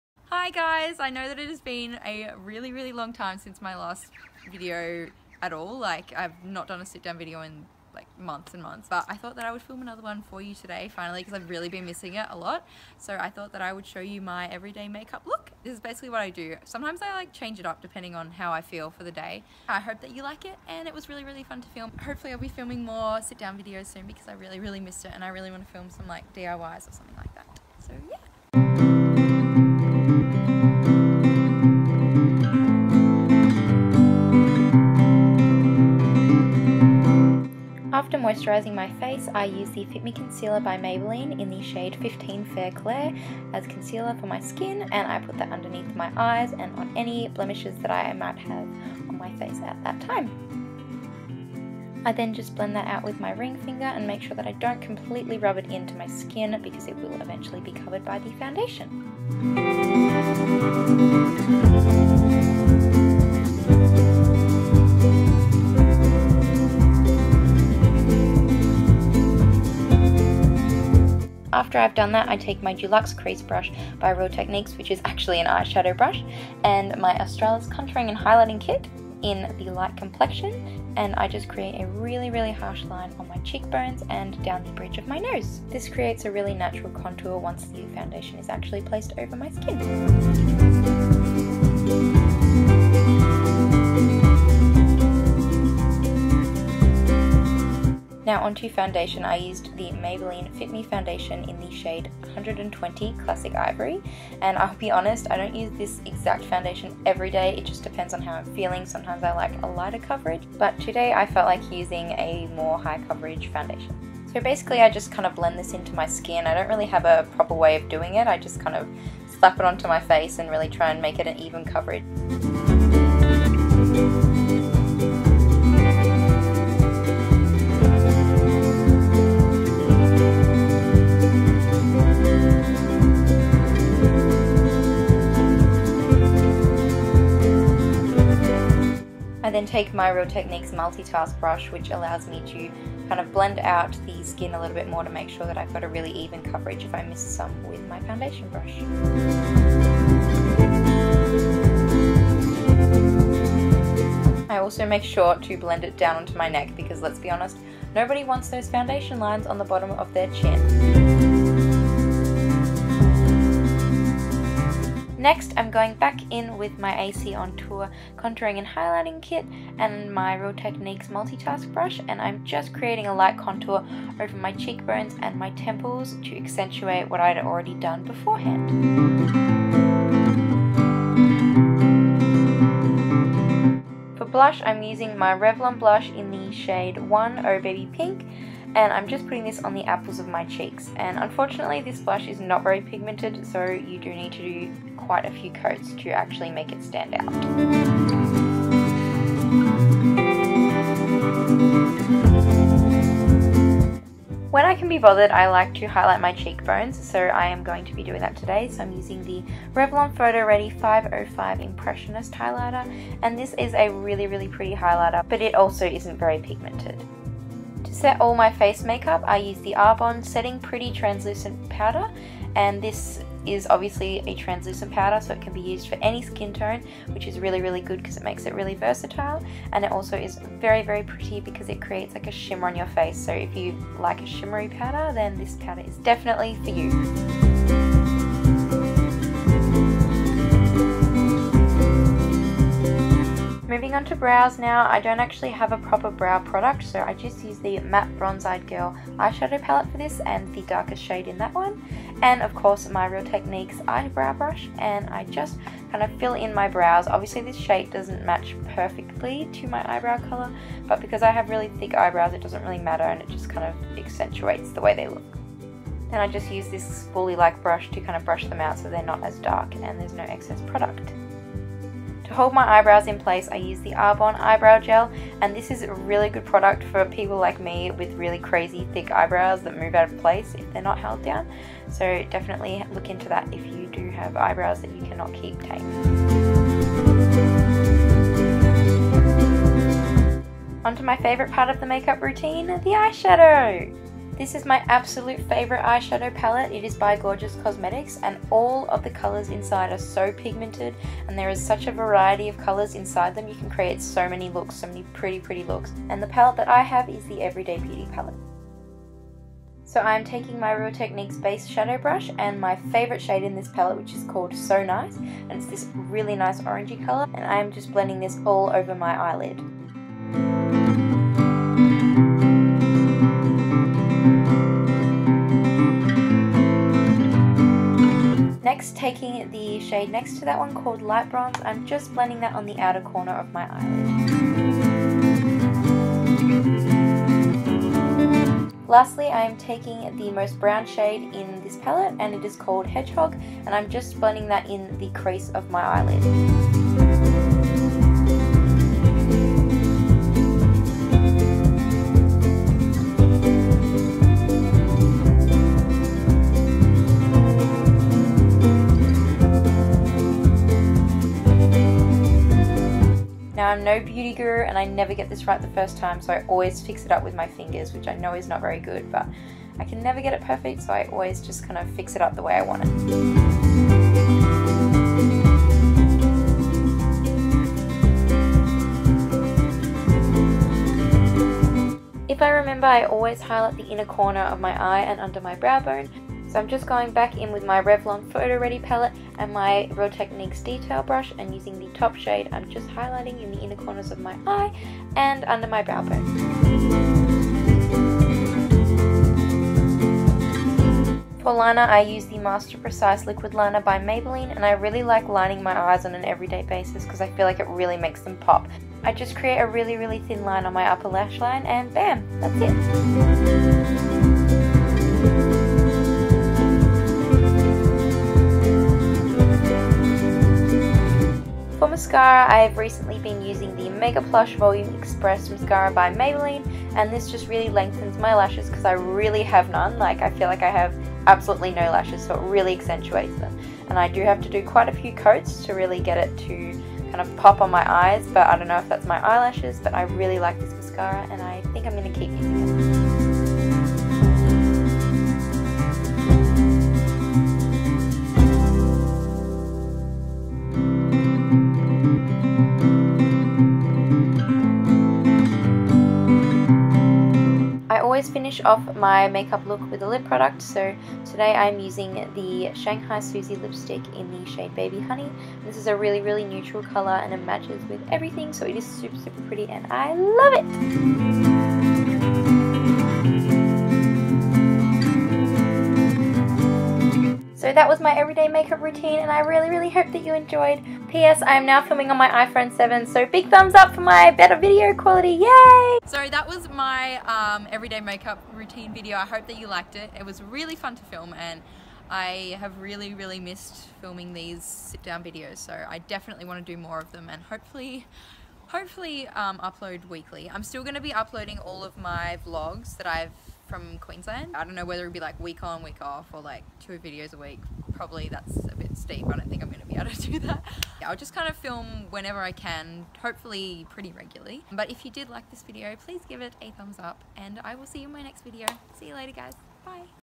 My everyday Makeup Routine!
Finally! There is another sit down video on my channel.

This is my everyday makeup routine!!

I would say I have two everyday makeup routines, there is this one and the ‘lazy girl’ makeup routine, so if you would like to see that too, I will be sure to do that for you guys!!

I am really happy with how this video turned out and hopefully you’re all seeing the better quality of videos I am displaying.

Products I used in this video:

Revlon Blush (001 Oh baby! Pink)

Stay Wonderful x

Casey’s Links